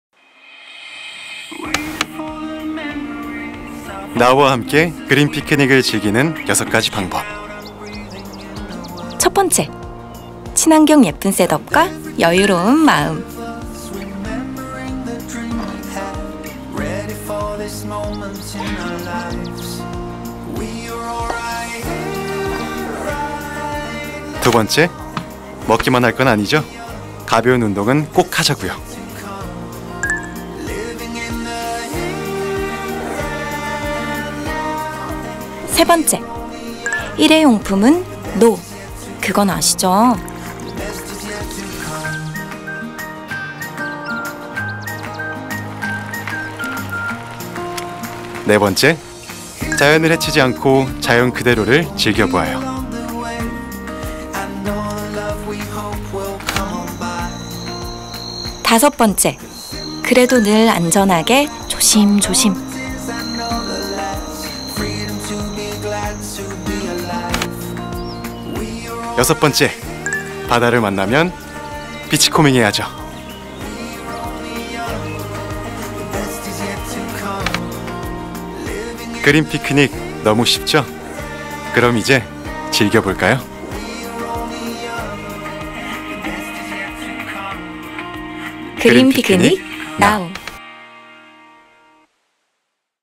(패션필름) 안효섭 연우의 그린 피크닉
‘나우’가 전속모델 안효섭, 연우와 함께한 패션필름 ‘Green Picnic’ 편을 공개했다.

이번 패션필름은 자연을 해치지 않고 있는 그대로 즐기고 돌아오자는 브랜드 가치관을 캠핑만큼은 어렵지 않지만 평범한 피크닉과는 다른 친환경 ‘그린 피크닉’ 컨셉으로 직관적이지만 유쾌하게 담아내고자 했다.

이를 위해 안효섭과 연우는 포틀랜드의 자연 친화적 라이프스타일을 배경으로 그린 피크닉을 즐기는 6가지 방법을 알려주는 가이드 역할을 맡았다.

브라운관 TV를 켜는 장면으로 시작하는 영상에서 두 모델은 숲 속에서 여유로운 마음가짐, 자연 그대로 안전하게 즐기기, 가벼운 운동과 일회용품 줄이기, 비치코밍 등을 제안하며 나우가 지향하는 친환경 아웃도어 컬처를 위트 있게 전달한다.

그린 피크닉을 위해 두 모델이 소화한 유니크한 디자인의 제품도 시선을 사로잡는다. 남녀공용의 우븐 스웻 셋업으로 연출한 커플룩부터 아웃포켓으로 포인트를 준 자켓과 조거 팬츠, 빈티지한 색감을 살린 다잉 반팔 티셔츠까지 런웨이 영상처럼 다양하게 등장한다.

이 제품들은 리사이클 나일론 폴리에스터, 공정무역 코튼, 가먼트다잉 등 친환경 소재와 공법이 적용돼 자연을 생각하며 스타일리시하게 입을 수 있어 그린 피크닉과 완벽하게 어우러진다.